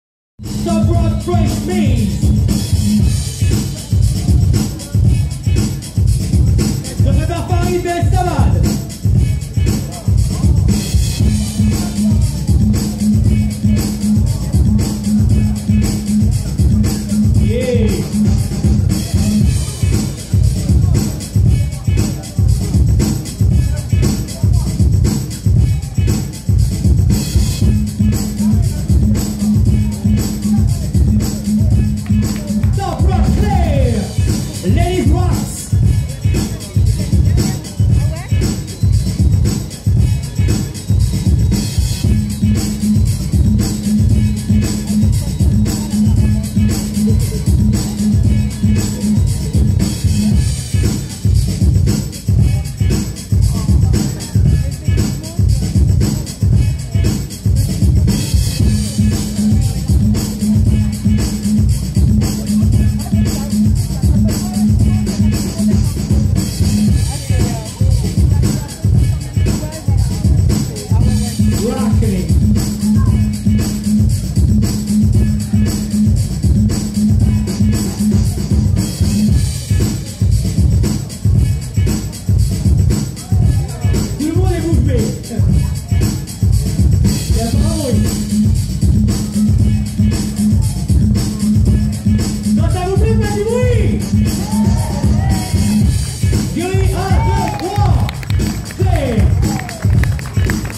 BATTLE DE LIZY 2016 1/4 TOP ROCK Clairea VS Fresmaze
Battle de lizy top rock
Winner Clairea